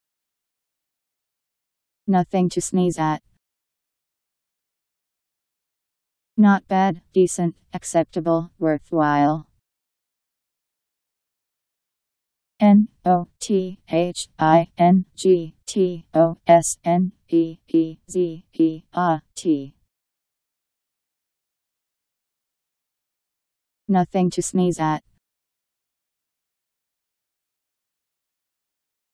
Nothing to sneeze at Meaning
Video shows what nothing to sneeze at means. Not bad; decent; acceptable; worthwhile..  Nothing to sneeze at Meaning. How to pronounce, definition audio dictionary. How to say nothing to sneeze at. Powered by MaryTTS, Wiktionary